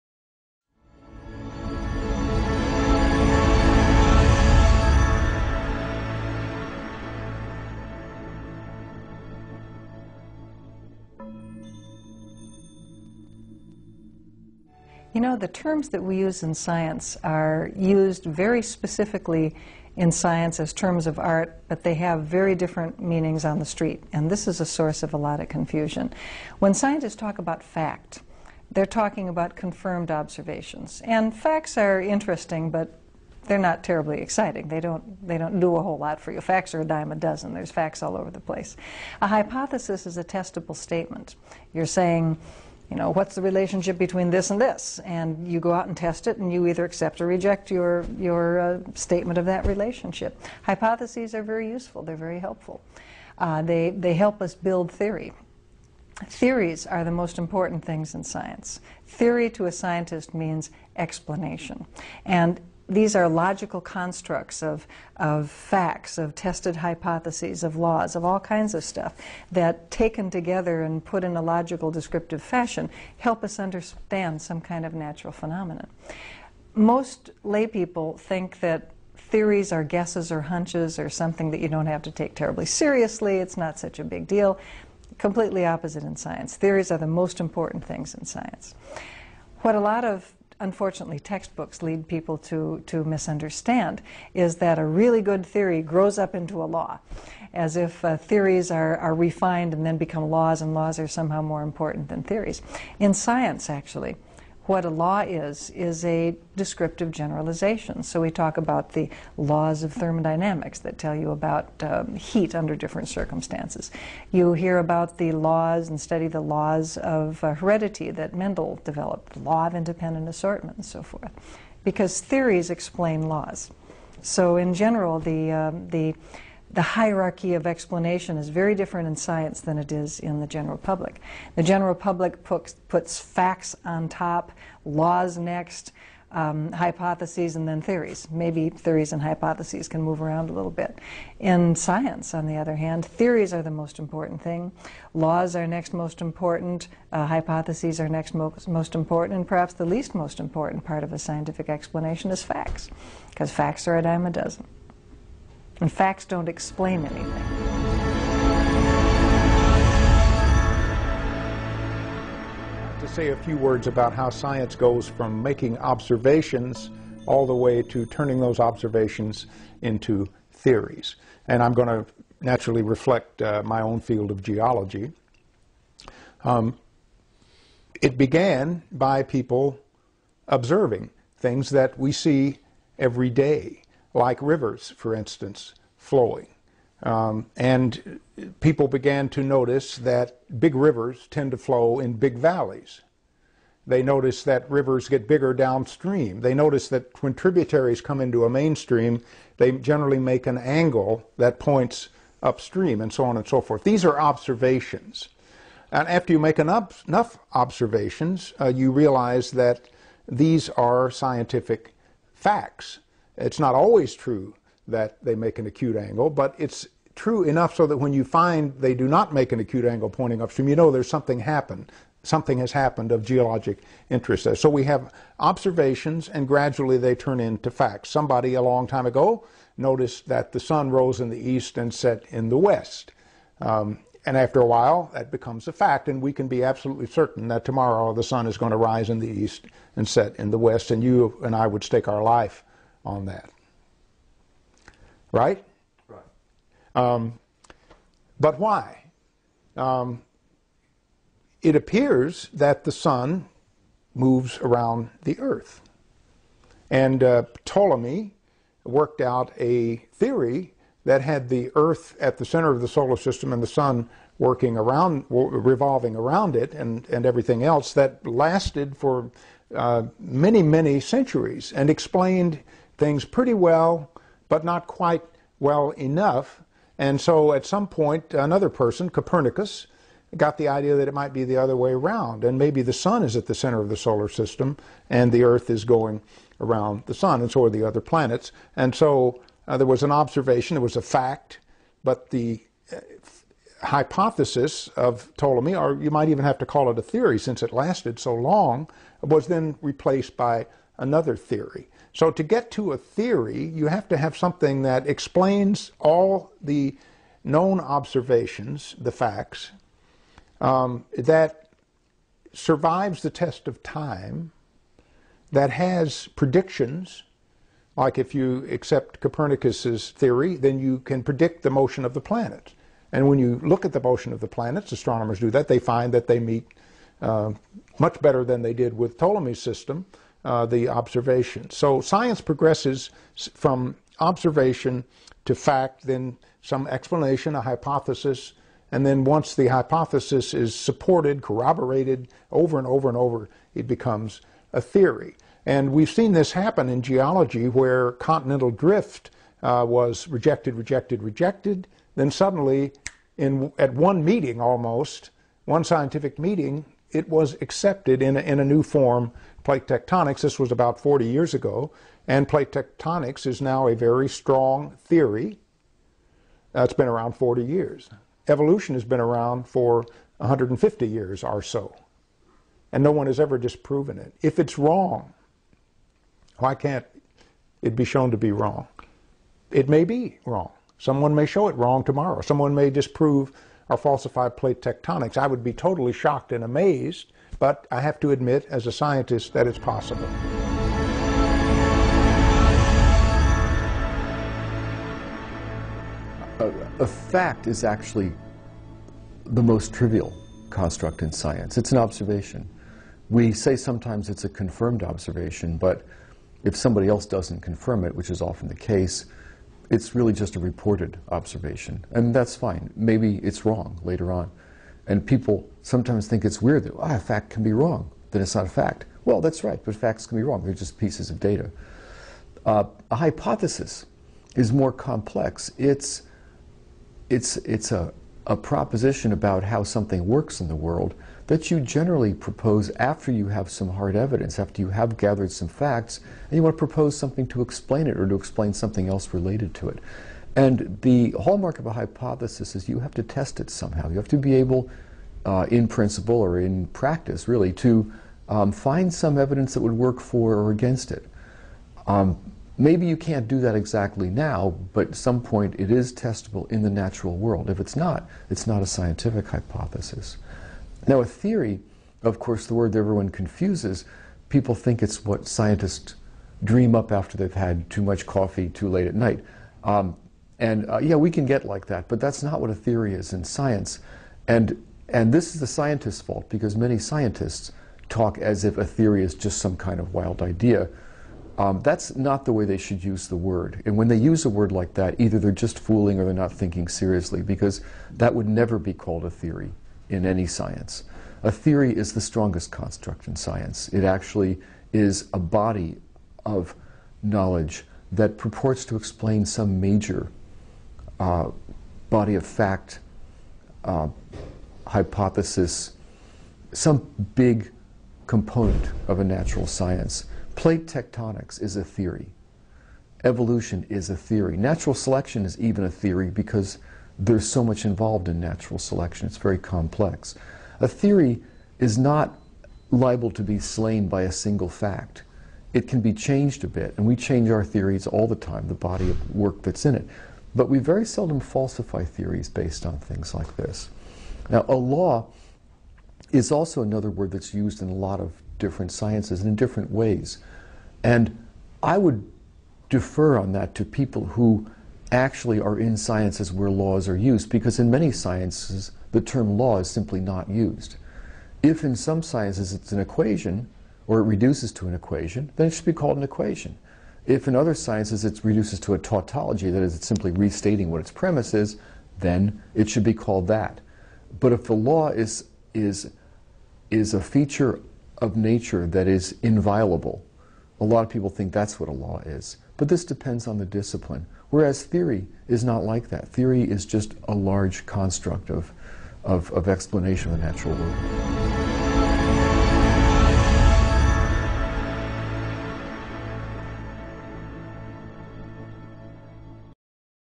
Explaining the Difference Between a Fact and Theory in Science
Eugenie Scott, James Powell and Kevin Padian explain the difference between fact and theory, and the place of laws in science.